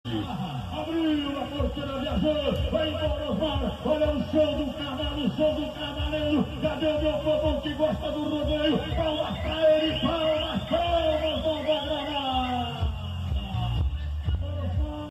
OSMAR ALVES DE OLIVEIRA X PANTER - RODEIO DE NOVA GRANADA - SP
Osmar Alves de Oliveira montando o animal Panter da Tropa do Juliano Verdi no Rodeio de Nova Granada - SP
Imagens: Bufalo Bill 📽